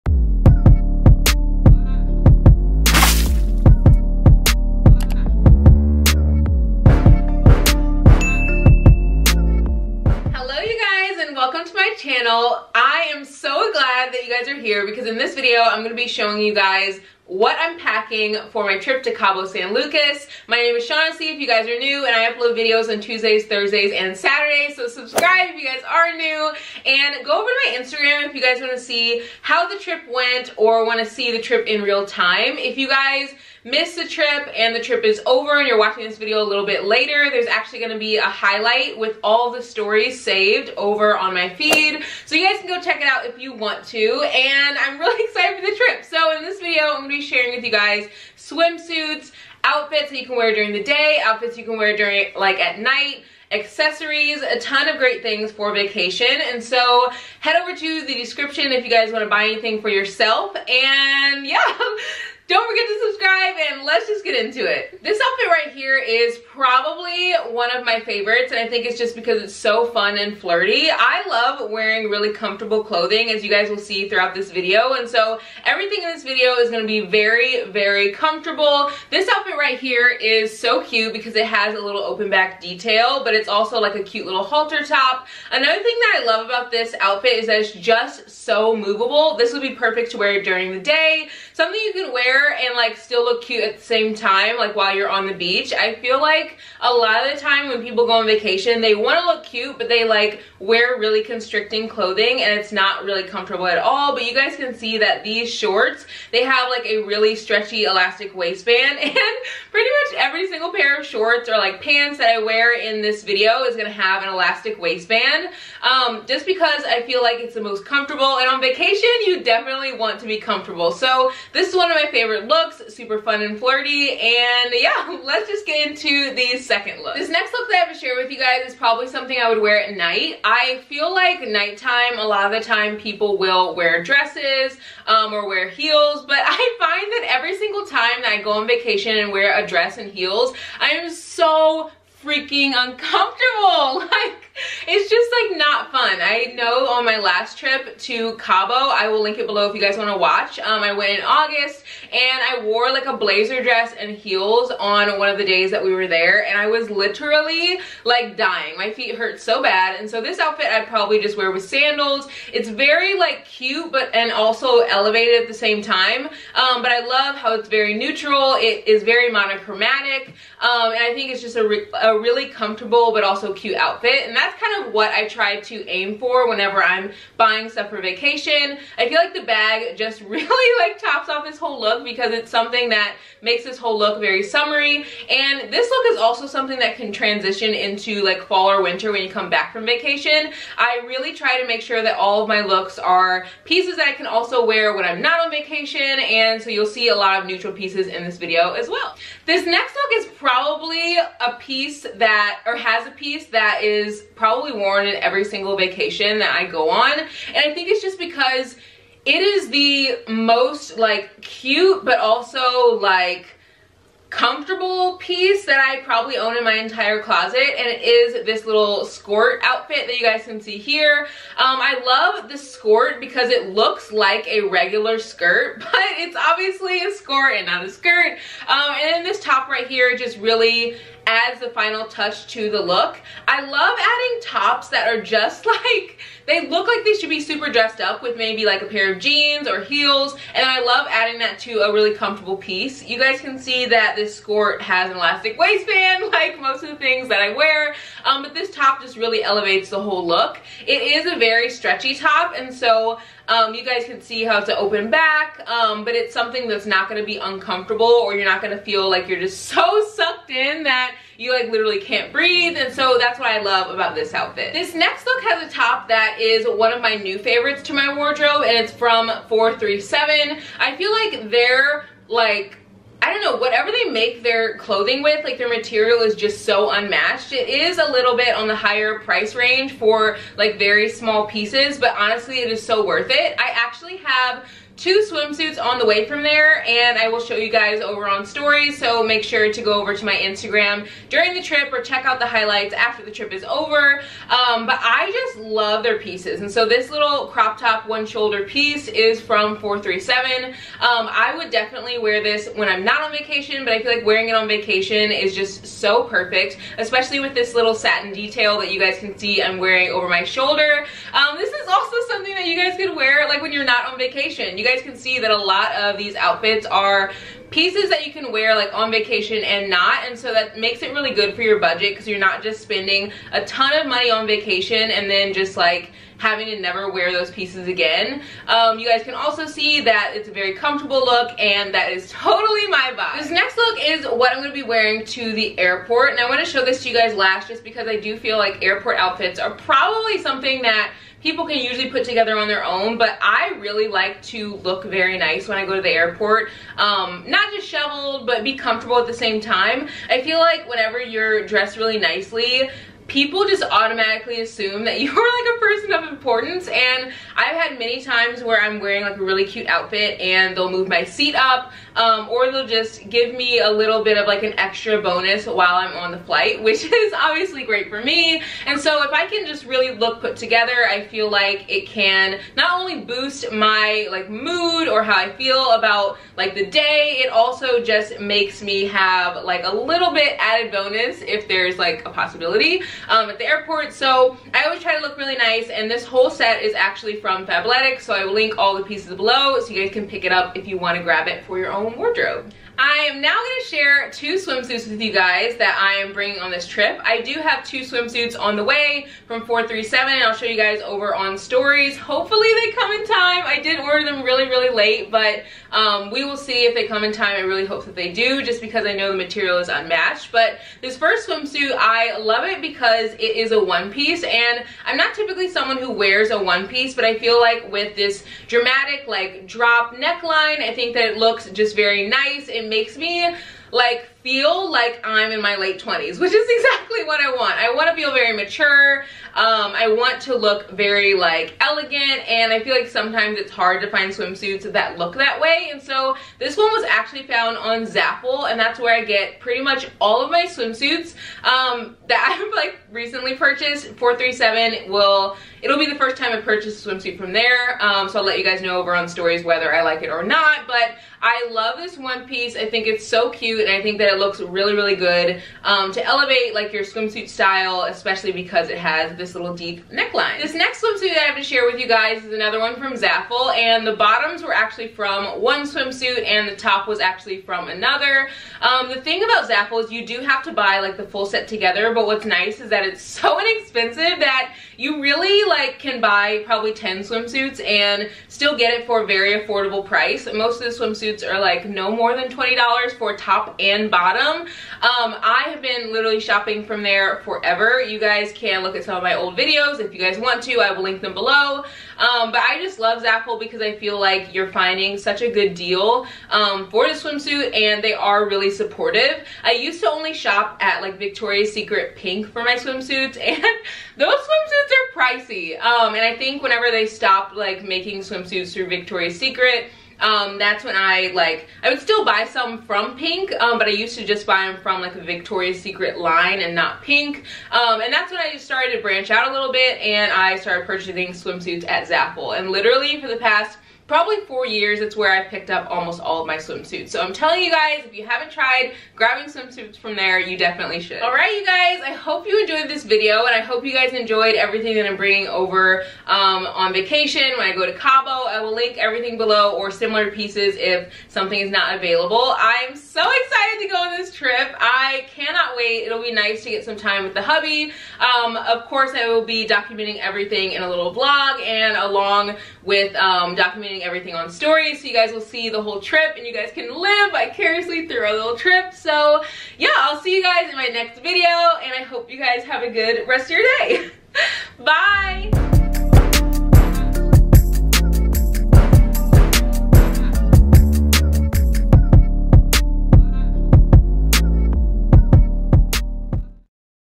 WHAT I’M PACKING FOR VACATION | CABO SAN LUCAS HOLIDAY OUTFITS
Beige Tank Top
Button Up

White One Shoulder Top

Black One Piece Swimsuit

Striped Bikini

Some of the products I wear or show are sent to me by the PR company. I will always give an honest review of the item. Unless disclaimed, I am in no way obligated to talk about or promote the item.